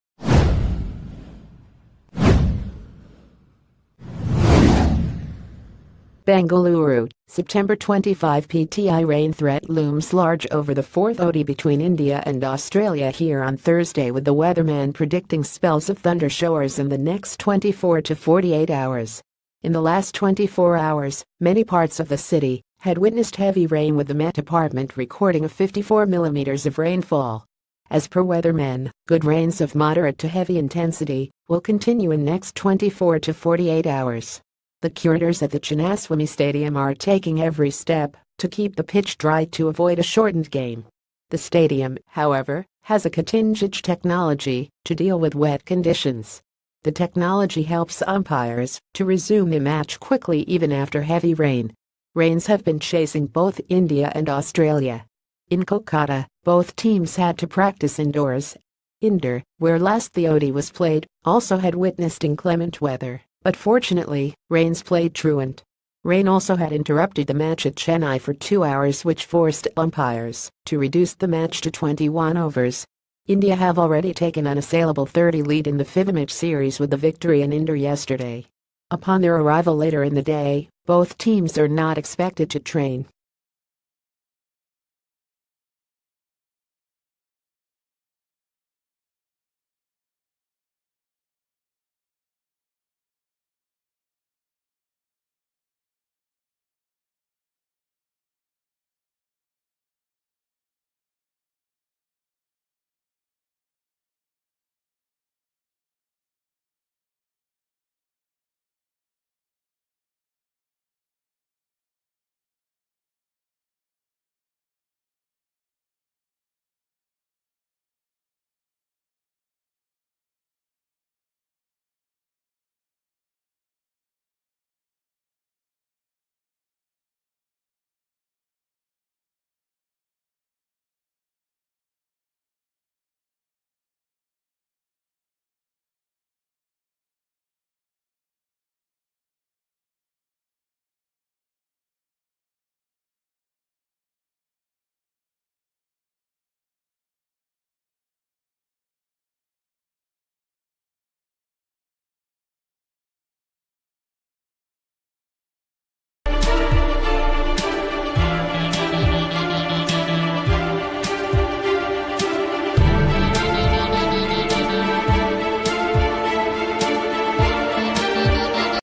Breaking News | Rain threat looms large over fourth odi
Rain threat looms large over fourth odi

Bengaluru, Sep 25 PTI Rain threat looms large over the fourth ODI between India and Australia here o...